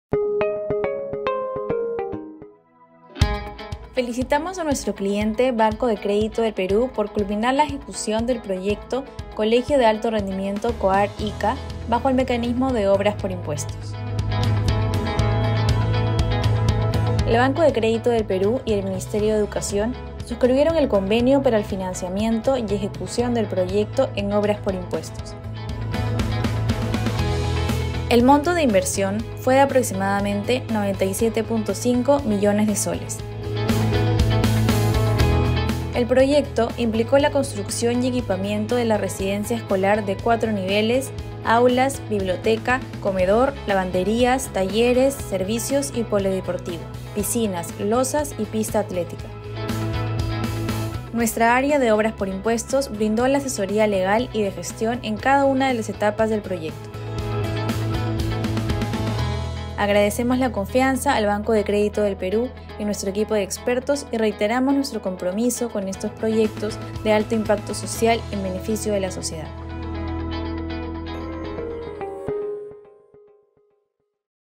We advised on the “Colegio de Alto Rendimiento - COAR ICA” Project by Banco de Crédito del Perú
We congratulate our client, Banco de Crédito del Perú (BCP), for the completion of their project "Colegio de Alto Rendimiento - COAR ICA" under the Works for Taxes mechanism.